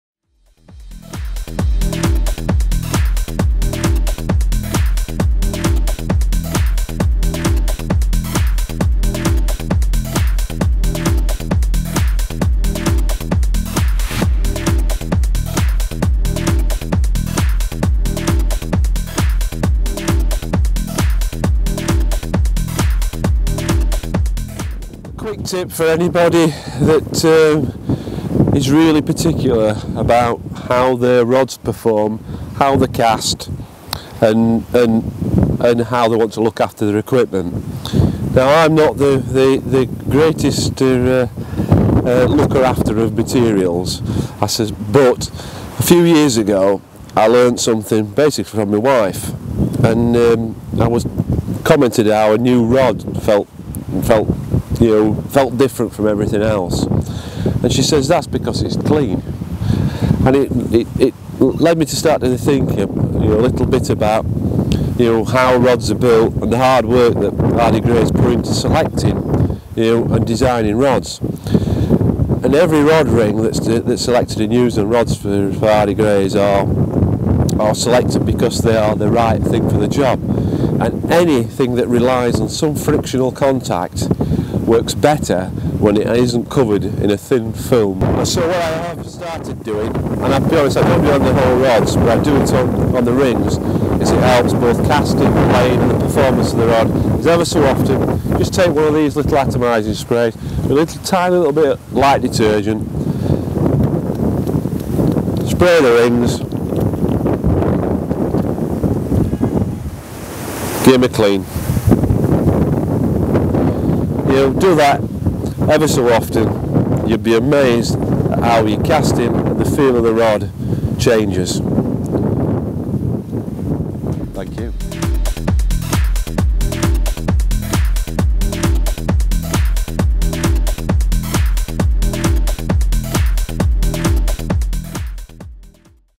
Tips from the Academy - Dave Moore
Dave Moore talks about how he maintains the performance of his rods.